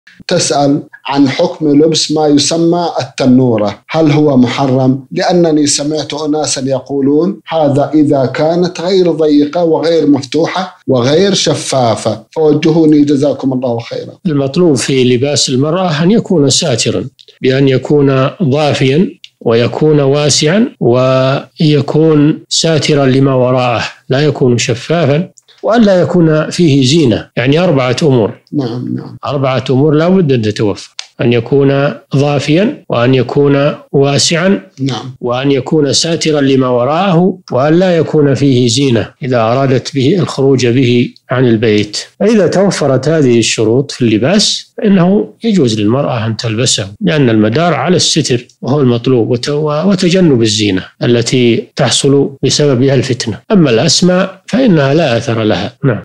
ما حكم لبس التنورة إذا كانت غير ضيقة وغير شفافة؟ الشيخ صالح بن فوزان الفوزان
نص السؤال: تسأل عن حكم لبس ما يسمى التنورة، هل هو محرم؟ لأنني سمعت أناساً يقولون: هذا إذا كانت غير ضيقة وغير مفتوحة وغير شفافة، فوجهوني؟
السيرة الذاتية :
للاشتراك في قناة الشيخ صالح الفوزان

رئيس الفريق
لمشروع كبار العلماء